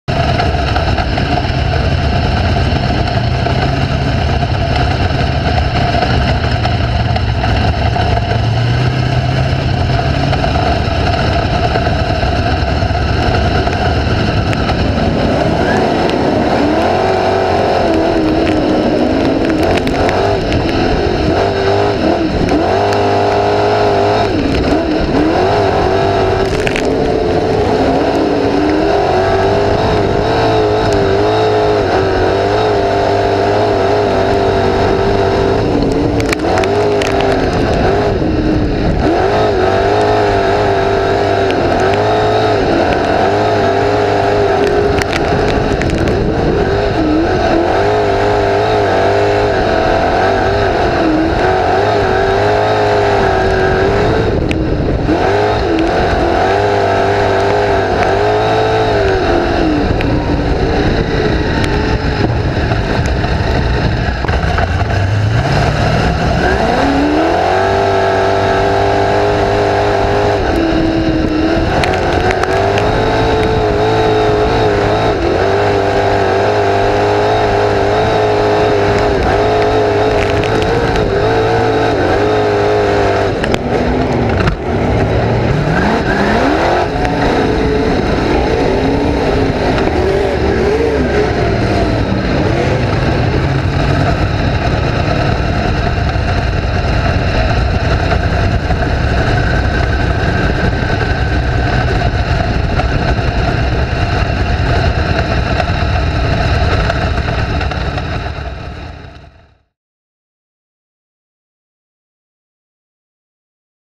Dave Steele East Bay Raceway Short Ride Along 12 7 13
East Bay Raceway Park - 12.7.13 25 Lap Feature Ended on Lap 4 Bent Radius Rod